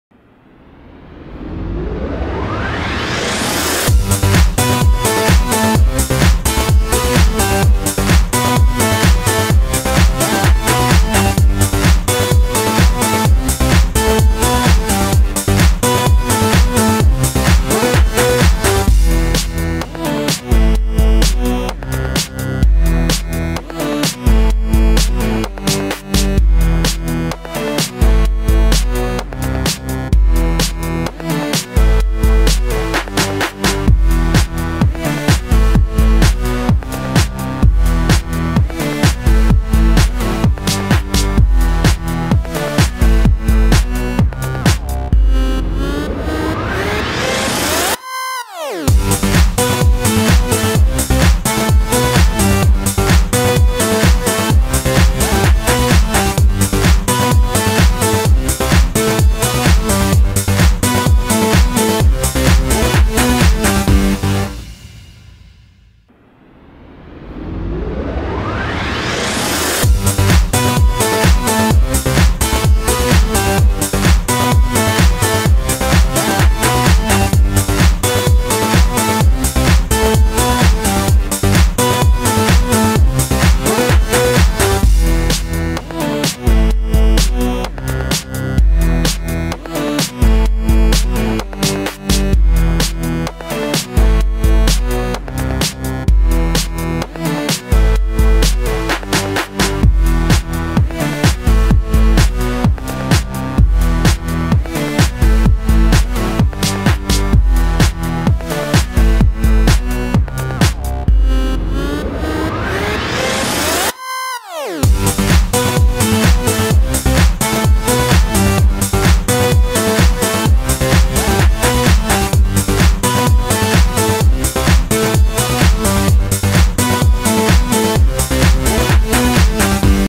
Everything Sucks! TV Series Review
This is my review of Everything Sucks! which aired on Netflix.